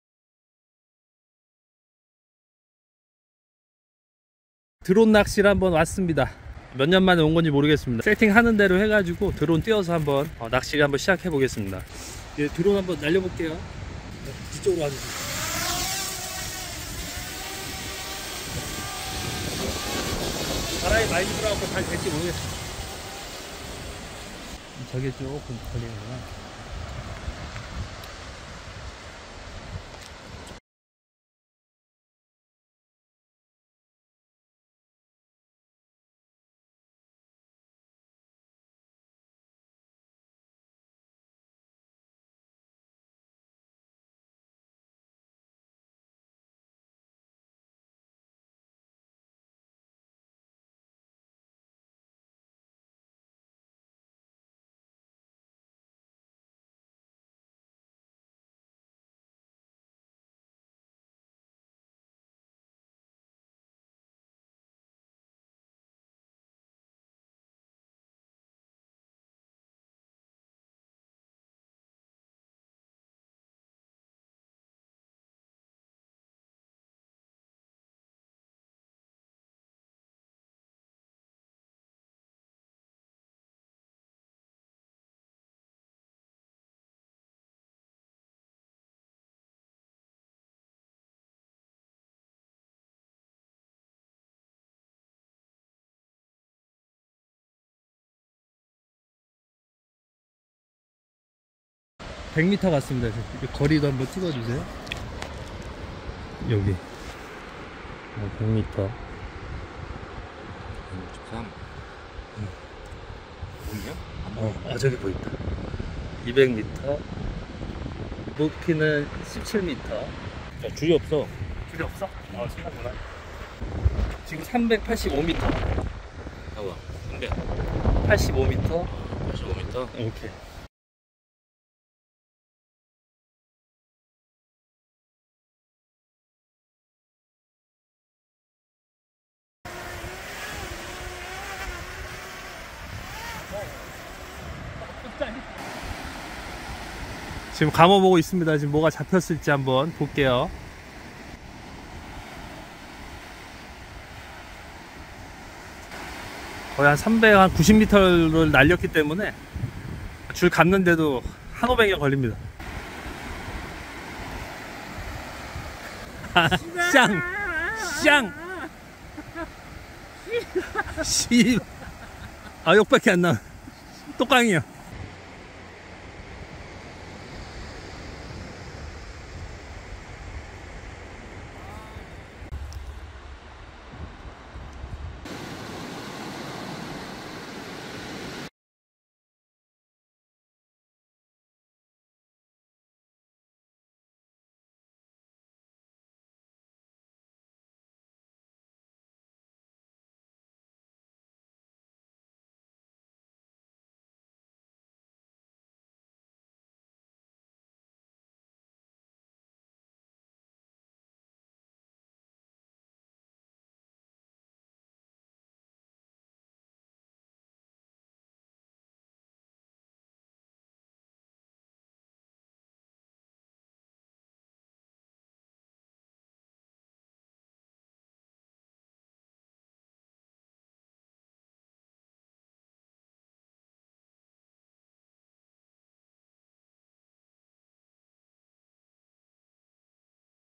매빅 미니로 드론 낚시 하러 왔다가 물고기 떼 발견했습니다. (feat : 매빅 미니) / mavic mini fishing
매빅 미니 드랍장치 만든지 1년이 넘었는데, 드디어 드론 낚시 하고 왔습니다.
바람은 좀 불었지만 매빅 미니 잘 버티고, 안전하게 낚시하고 돌아 왔습니다.